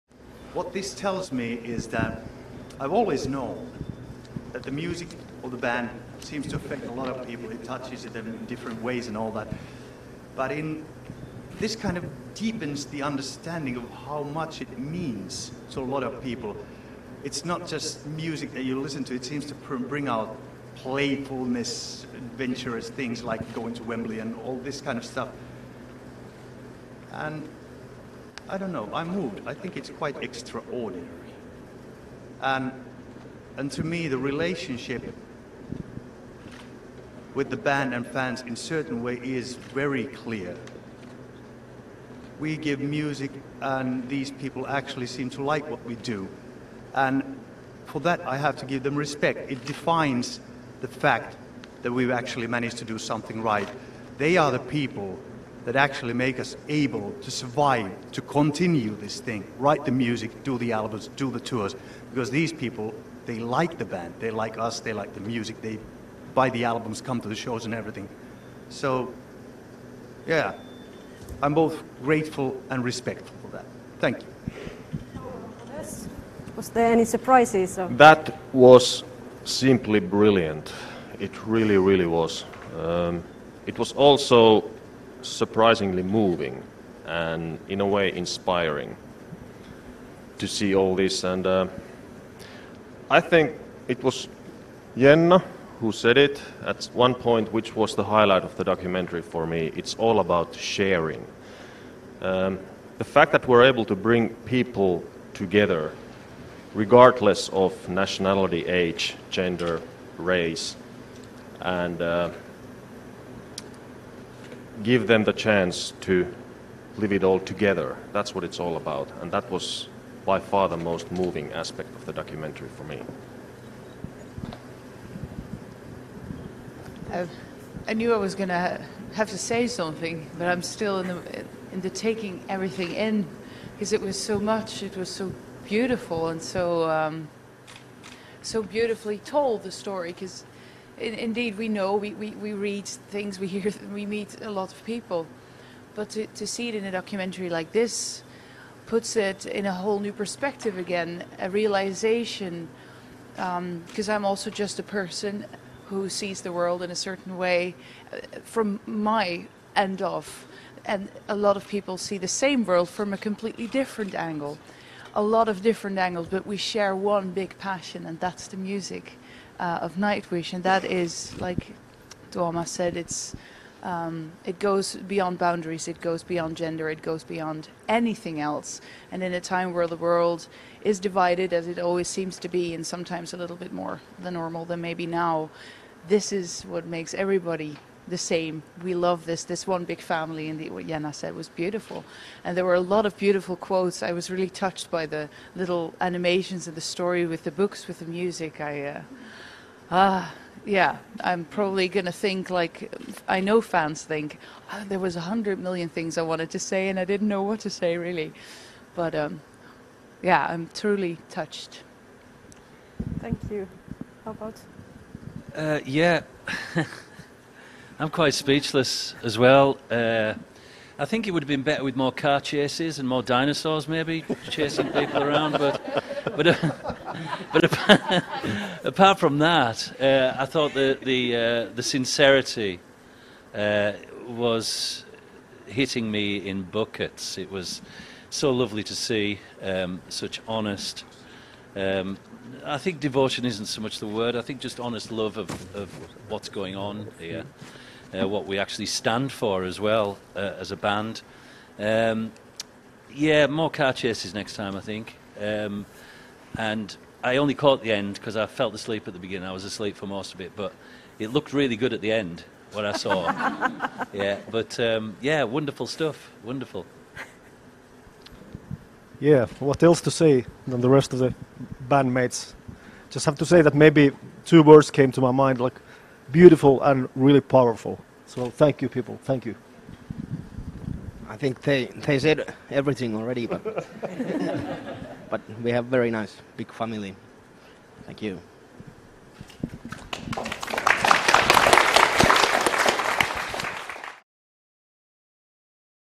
Premiere of To Nightwish with Love documentary
Tuomas Holopainen, Marco Hietala, Floor Jansen, Emppu Vuorinen, Kai Hahto and Troy Donockley are talking about To Nightwish with Love documentary